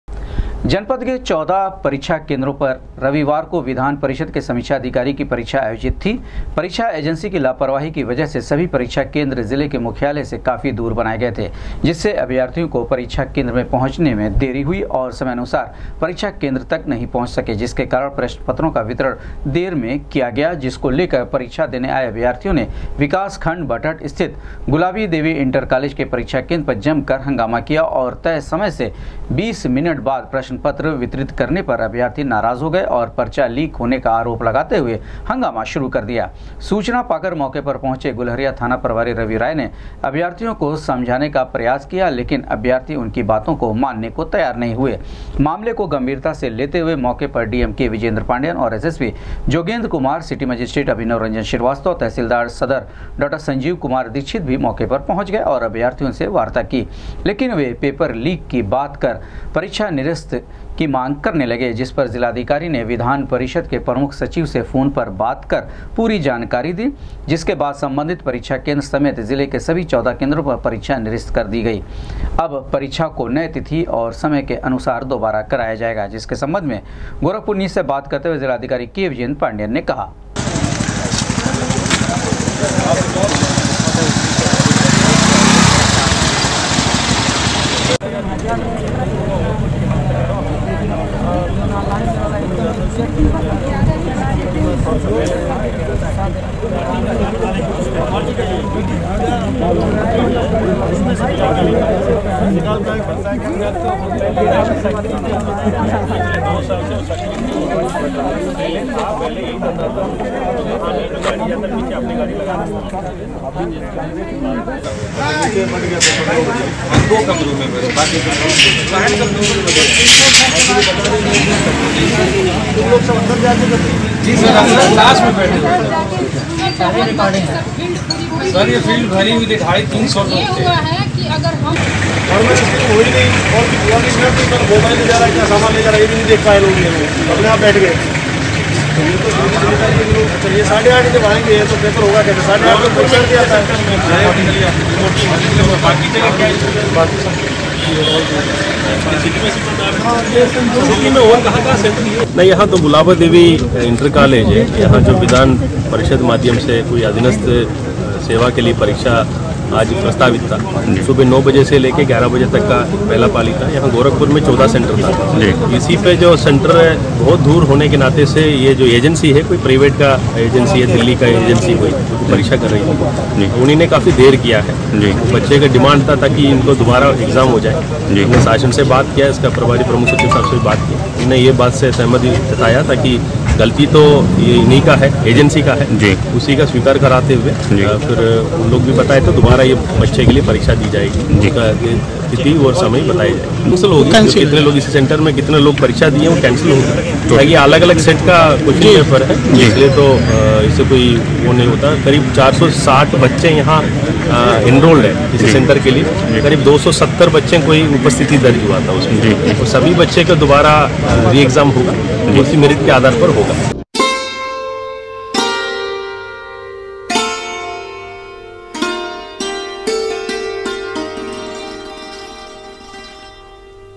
विधान परिषद के समीक्षा अधिकारी की परीक्षा के पर्चा लीक का आरोप लगा कर अभ्यर्थियों ने किया जमकर
भटहट स्थित परीक्षा केंद्र पर विधान परिषद के समीक्षा अधिकारी की परीक्षा के पर्चा लीक का आरोप लगा कर अभ्यर्थियों ने किया जमकर हंगामा ,मौके पर पहॅुचे डीएम और एसएसपी ने स्थिति को सँभाला, परीक्षा किया निरस्त,डीएम ने कहा, अब दोबारा होगी परीक्षा।।।